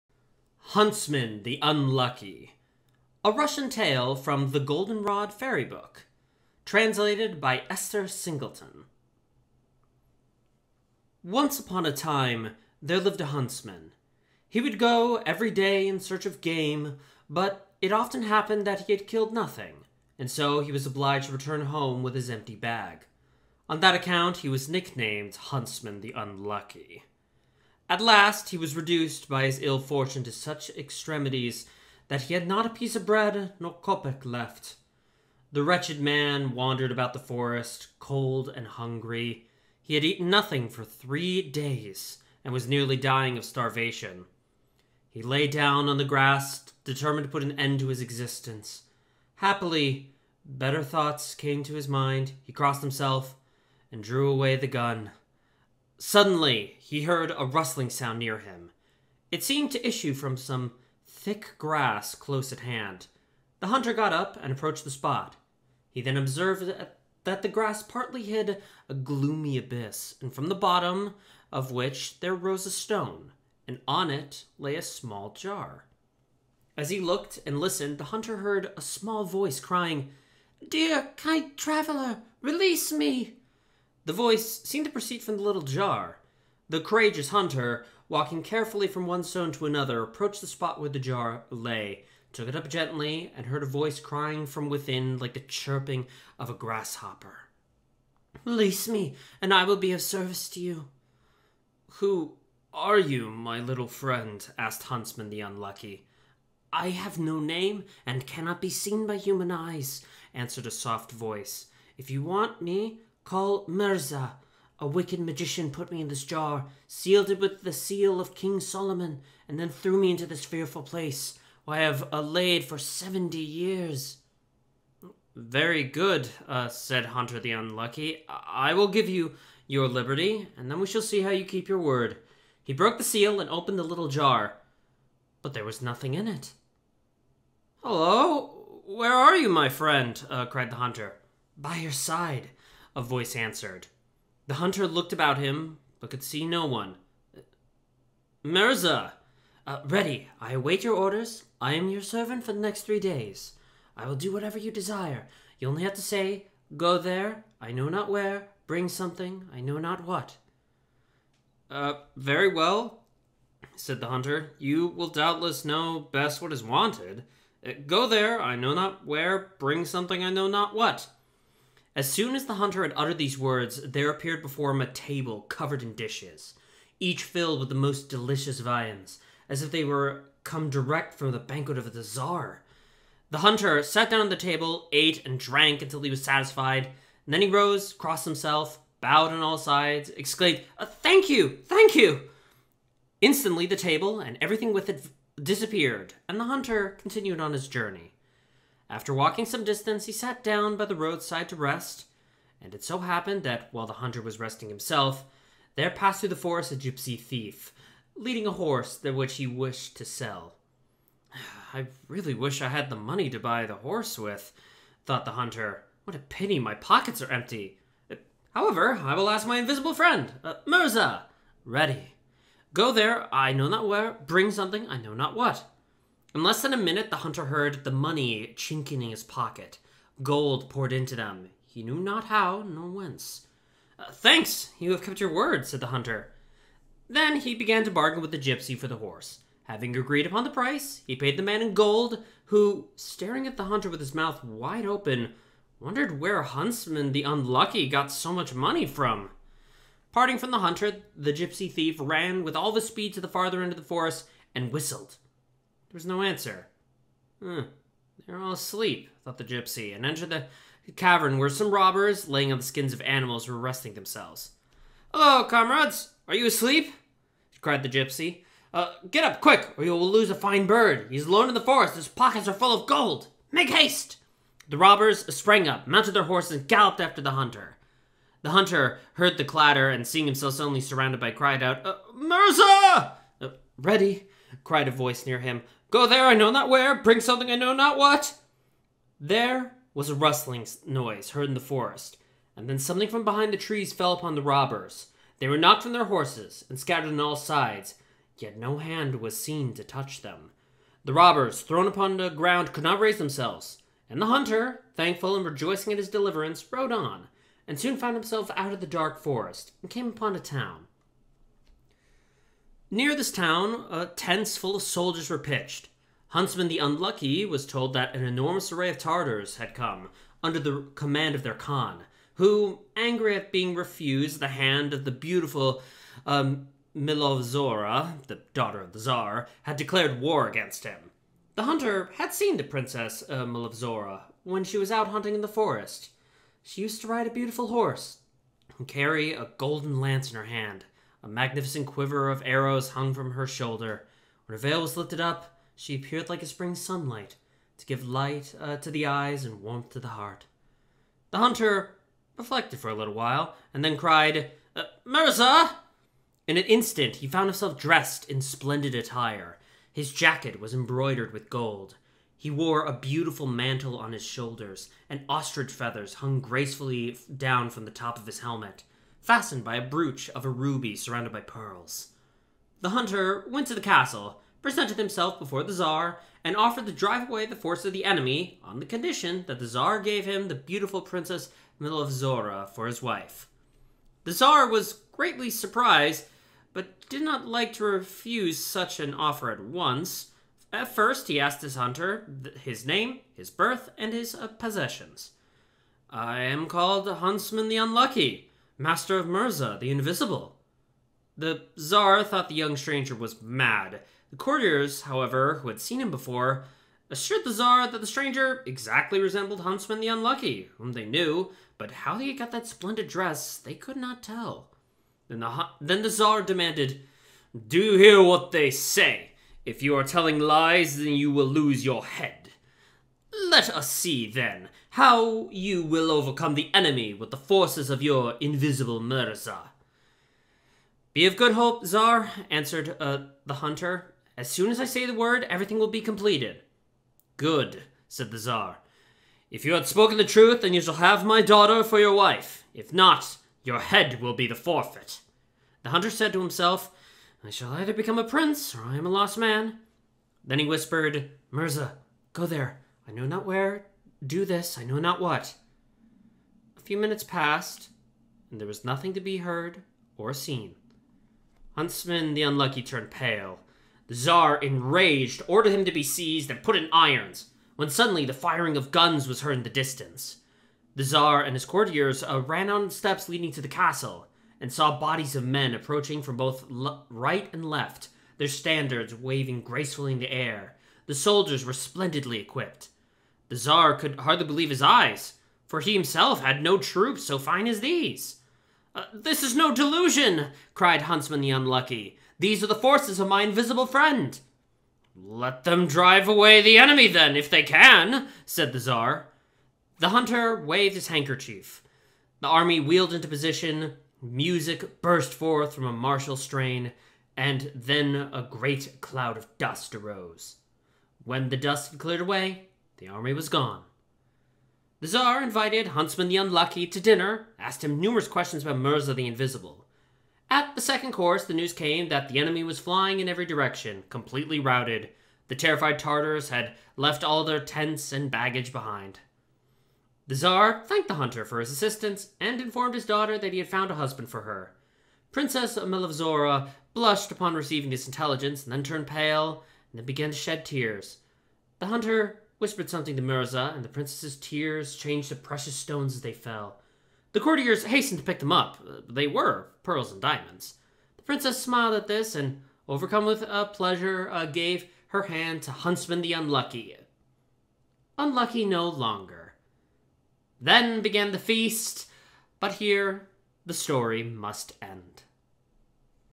Huntsman the Unlucky
From 1903: the Golden Rod Fairy Book, a collection of many tales translated by Esther Singleton. Today's story is Huntsman the Unlucky, from Russia!

That sure was an eventful three days huh?